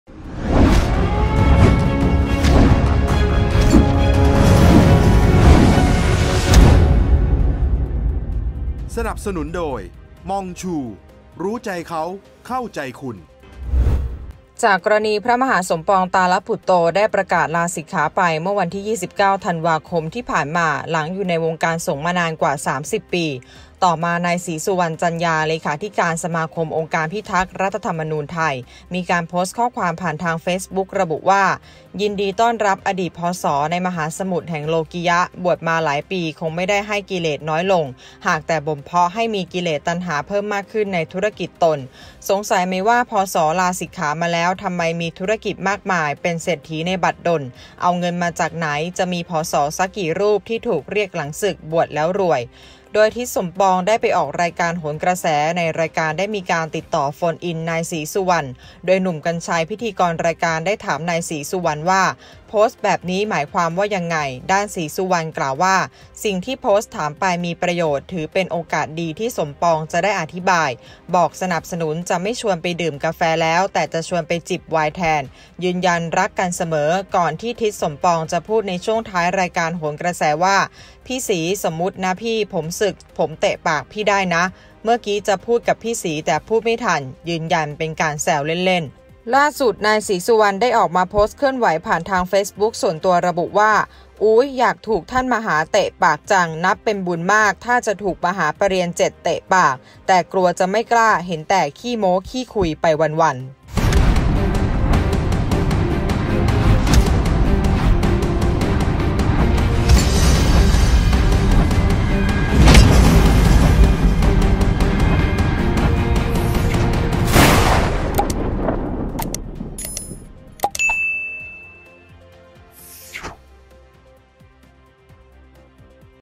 ใส่กันคนละดอก! "ศรีสุวรรณ" สวนกลับ "ทิดสมปอง" อยากโดนมหาเปรียญ 7 เตะปากจังเลย | ข่าวด่วน | TOP NEWS
นายศรีสุวรรณ ได้ออกมาโพสต์เคลื่อนไหวผ่านทางเฟซบุ๊กส่วนตัว ระบุว่า "อู้ยย อยากถูกท่านมหาเตะปากจัง นับเป็นบุญมาก ถ้าจะถูกมหาเปรียญ 7 เตะปาก แต่กลัวจะไม่กล้า เห็นแต่ขี้โม้ขี้คุยไปวันๆ"